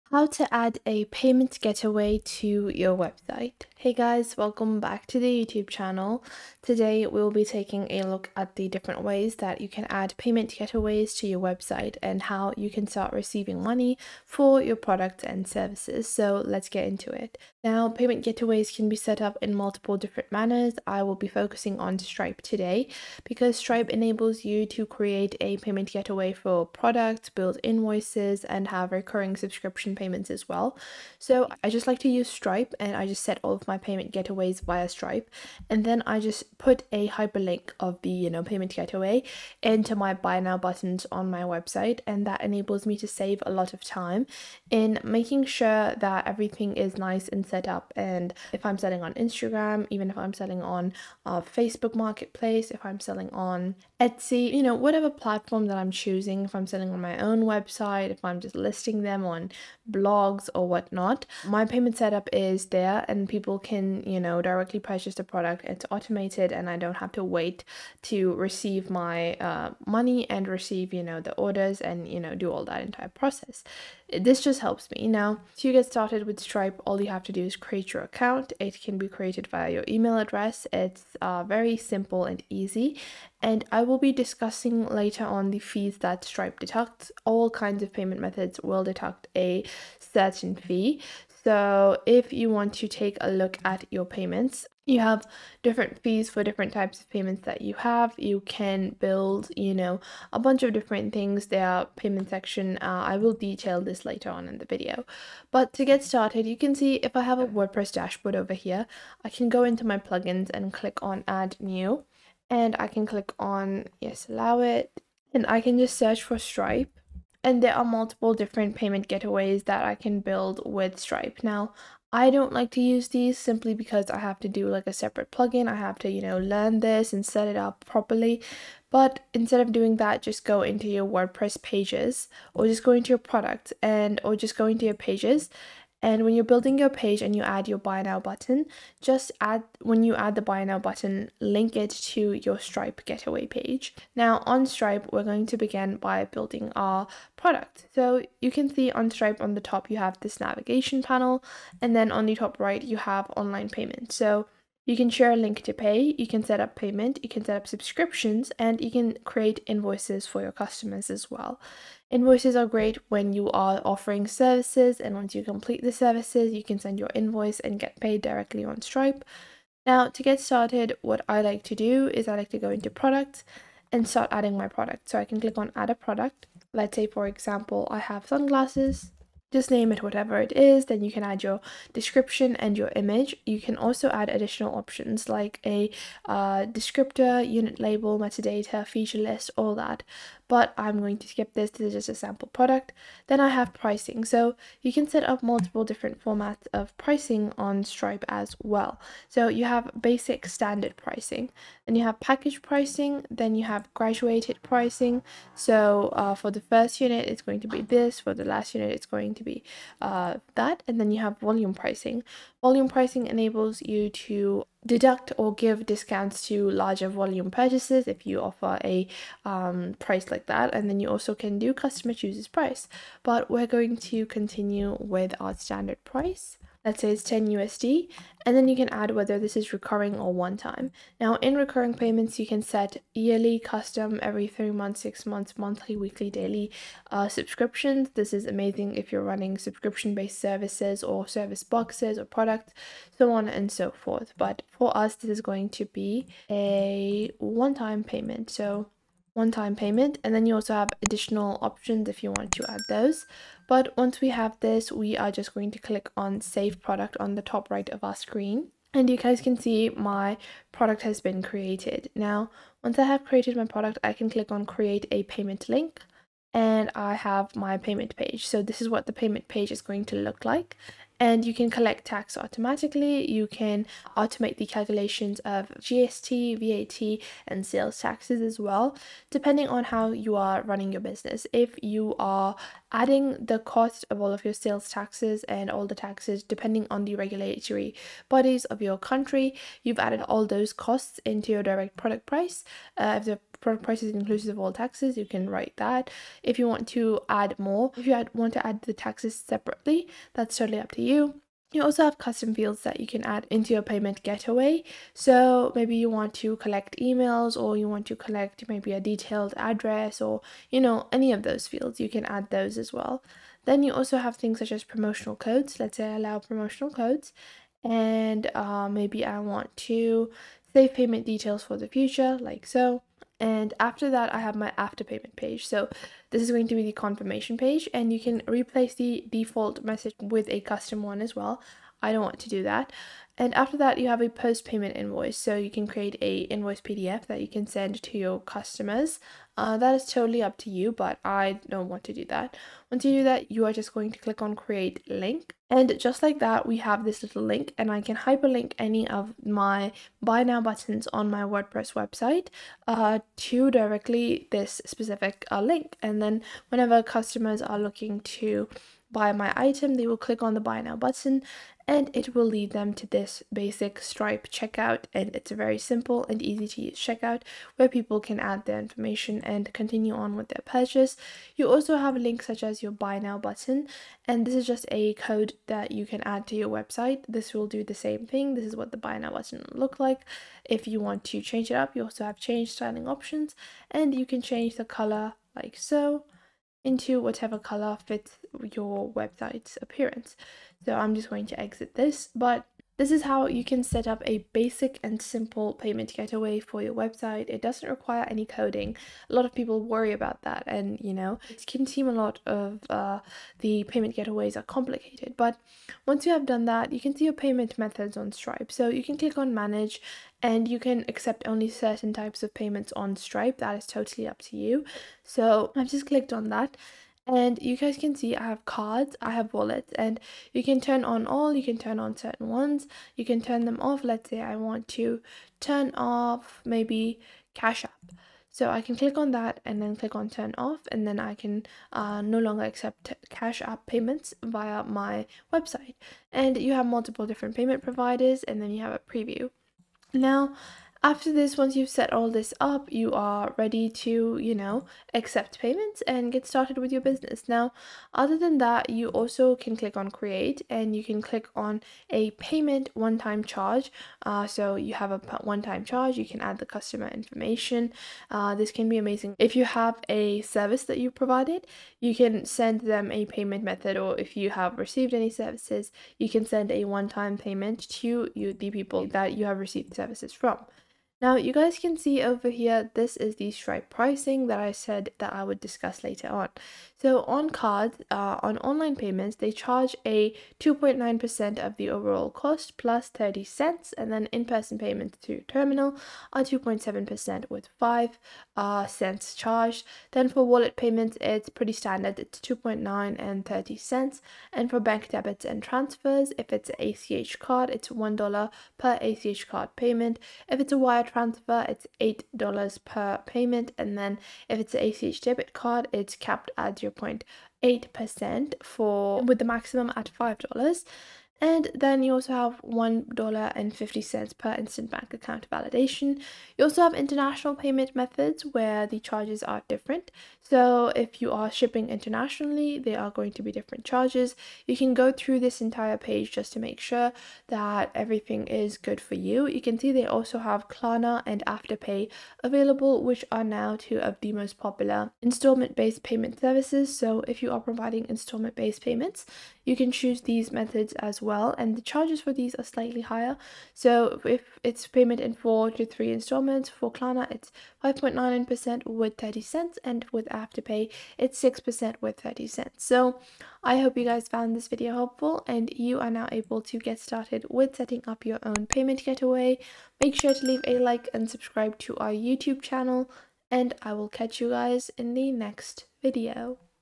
How To Add A Payment Gateway To Your Website
In this video I show you How To Add A Payment Gateway To Your Website. It is really easy to do and learn to do it in just a few minutes by following this super helpful tutorial to get a better understanding of everything.